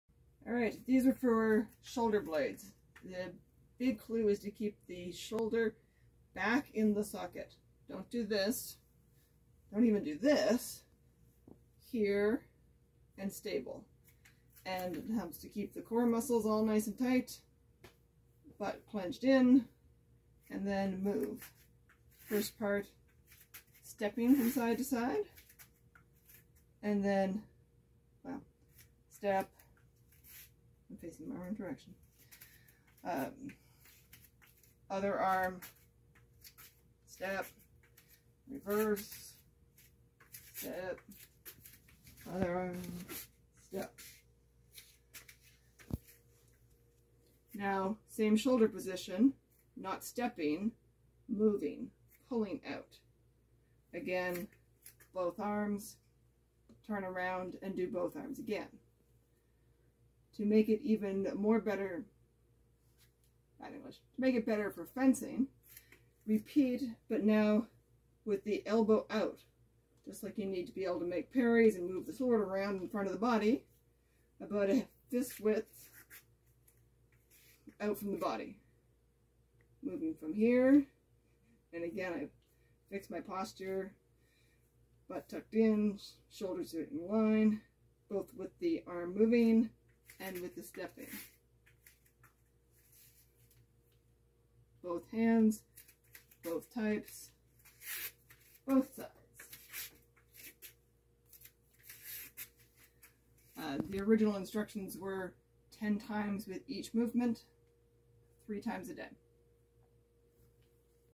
shoulder and back exercises
Excercises prescribed by physiotherapist to strrengthen shoulder muscles and train shoulder to keep the ball back in the socker.